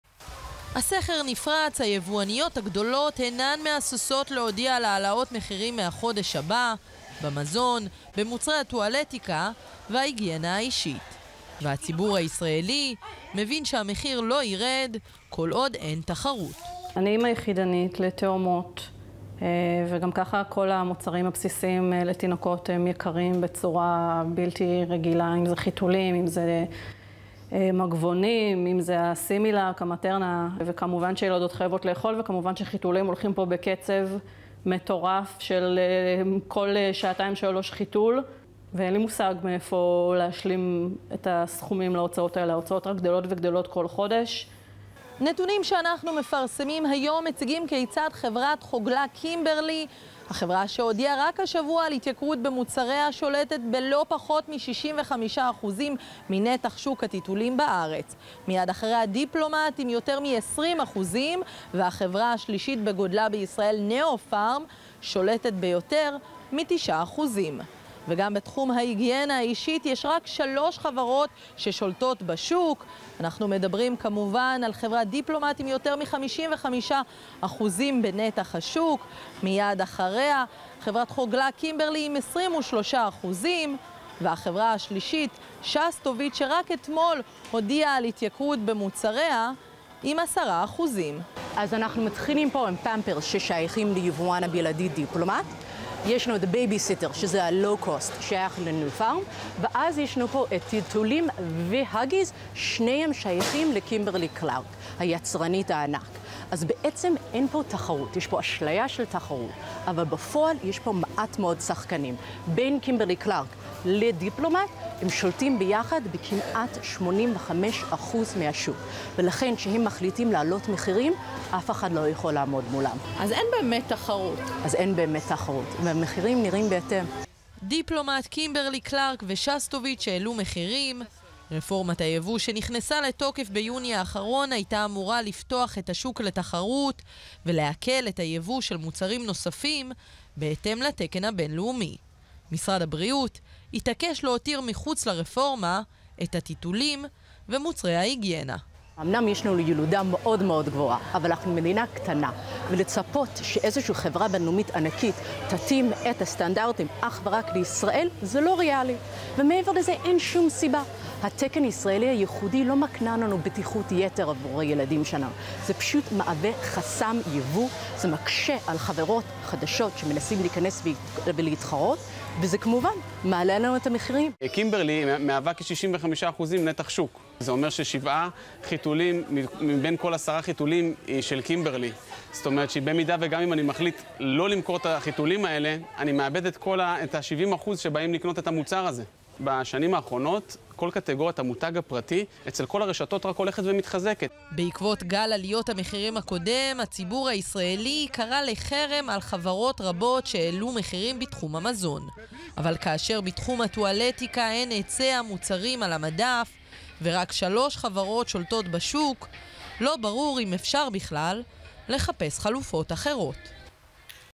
יקר פה: המונופול בשוק החיתולים והרפורמה שסוכלה
כאן חדשות | שתי יבואניות ענק - קימברלי ודיפלומט, שולטות ב-85% משוק החיתולים. עם כזו ריכוזיות, ליצרנים הקטנים אין ברירה אלא ליישר קו כשהגדולות מחליטות על העלאת מחירים. רפורמת היבוא הוחרגה מהתחום - בהוראת משרד הבריאות. הכתבה של דנה ירקצי, מתוך חדשות הערב 20.07.22